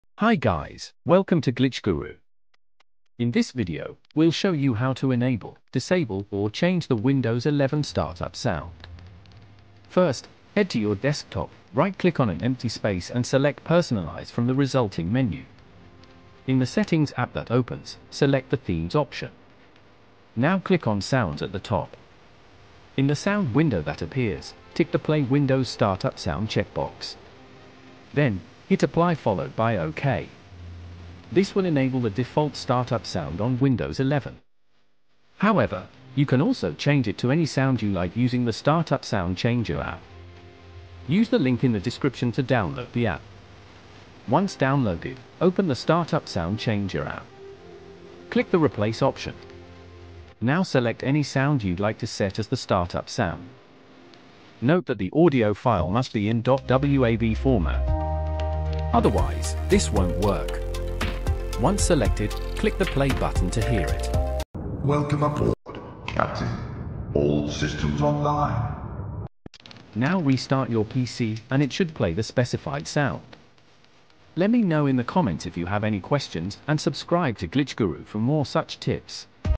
How to Enable, Disable or Change Windows 11 Startup Sound
Here's how to enable, disable, or change the startup sound on your Windows 11 computer or laptop.